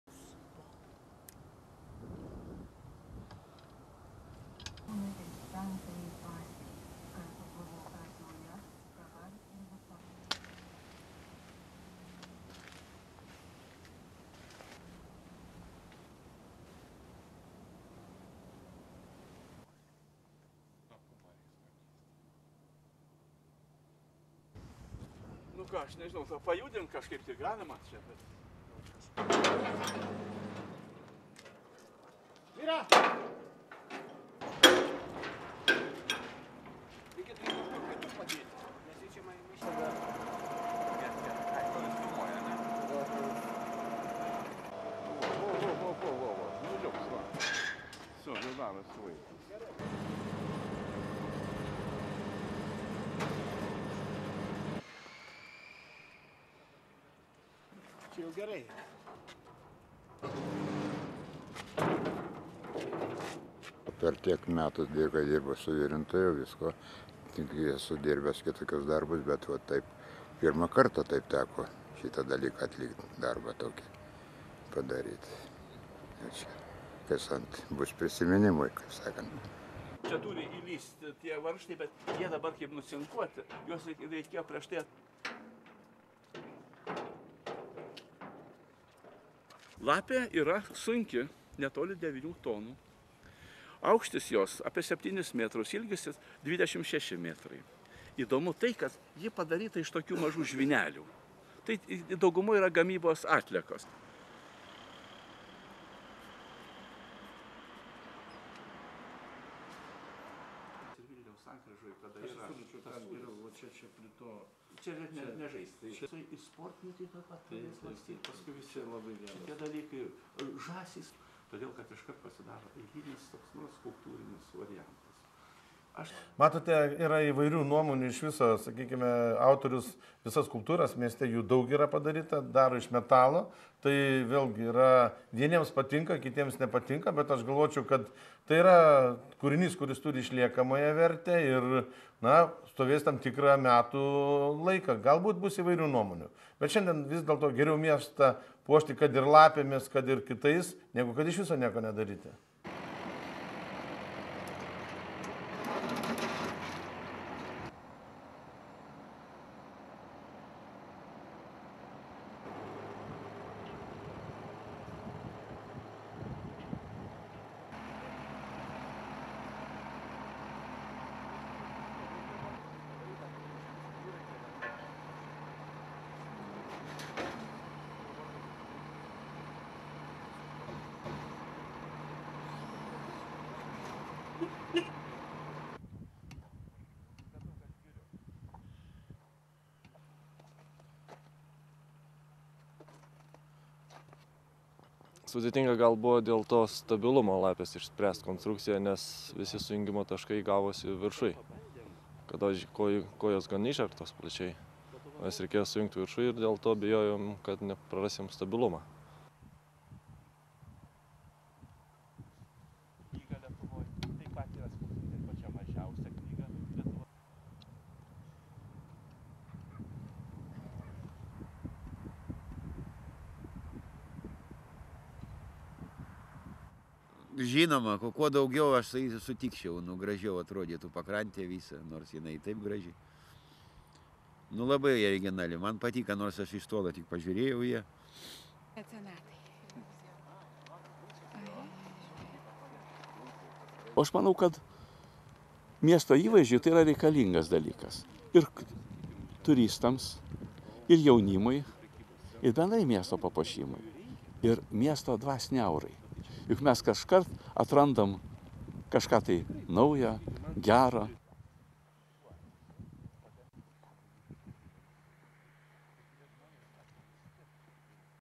A cunning fox to entice tourists
Siauliai, Lithuania - 2 December 2009
1. Wide of Hill of Crosses at sunrise
2. Wide of crosses
3. Mid of statues with crosses hanging
4. Close up same
5. Wide of Hill of Crosses
6. Wide of Vilius Puronas near the parts of his fox statue laying on the ground
7. Close up worker attaching cables to a part of the statue
8. Wide of workers dragging part of a statue
9. Wide of crane
10. Mid shot part of the fox head being lifted by crane
11. Wide of workers pulling part of head down
12. Close up worker bringing two parts of statue together
13. Wide of same
14. SOUNDBITE (Lithuanian) Vaclovas Butkus, welder from Elga Metal Plant:
"I have been a welder for many years, but it is the first time I have had such a job. I will remember it for a long time."
15. Mid of Vilius Puronas near part of his statue
16. Close up metal plates on the statue''s surface
17. SOUNDBITE (Lithuanian) Vilius Puronas, sculptor and designer, author of the fox statue:
"The fox is quite heavy, it weighs about 9 tons. It is 7 metres high and 26 metres long. What is interesting about it, is that it is made of small pieces of metal, which are mainly wastes of metal production."
18.Wide of fox statue designer, Vilius Puronas, entering the Siauliai City Administration building,
19. Close up city map
20. Close up Siauliai Mayor Genadijus Miksys looking at the map
21. Wide of fox statue designer, Vilius Puronas, and Siauliai Mayor Genadijus Miksys looking at the map
22. SOUNDBITE (Lithuanian) Genadijus Miksys, Mayor of Siauliai:
"There are different opinions (about this statue), this artist (Vilius Puronas) has made various sculptures - some people like them, some don''t. But I think that this statue has a certain significance for the future. It will be standing there for a long time. There could be different opinions about it, but I think it is better to decorate the city with fox statues and other sculptures, rather than do nothing at all."
Siauliai, Lithuania - 22 December 2009
*NIGHT SHOTS*
23. Wide shot statue of the giant fox being driven on a truck in the street
24. Close up police car
25. Back mid shot statue being driven
26. Mid shot workers pulling cables to lift the statue
27. Wide of statue being lifted
28. Close up workers put the statue down
29. Wide of statue
Siauliai, Lithuania - 23 December 2009
30. Wide of fox statue on the shore of the lake
31. Mid shot statue engineer Viktoras Jancenkovas looking at the statue
32. SOUNDBITE (Lithuanian) Viktoras Jancenkovas, engineer:
"There were some difficulties with regard to the statue''s stableness. We needed to make some constructive decisions. All the conjunction points are very high, at the top of the statue. We spread the legs (of the statue) far from each other, so that the statue would be more stable."
33. Mid of statue
34. Wide of ice fishermen seen through legs of statue
35. Wide of fishermen seated on ice on the lake
36. Close up float of a fishing rod in the water
37. SOUNDBITE (Lithuanian) Vladimiras Ivanovas, ice fisherman, voxpop:
"I think that the more decorations there are on the shore (of the lake) - the better. Although the shore is beautiful by itself, I think the fox is a very original statue. Although I''ve seen it only from a distance, I like it."
38. Wide of Puronas talking to journalists
39. SOUNDBITE (Lithuanian) Vilius Puronas, sculptor and designer, author of the fox statue:
40. Wide of statue
41. Mid of fox''s face
LEAD IN :
A giant fox is stalking an icy lakeside in Lithuania.
It''s hoped the cunning beast will lure tourists to the northern city of Siauliai.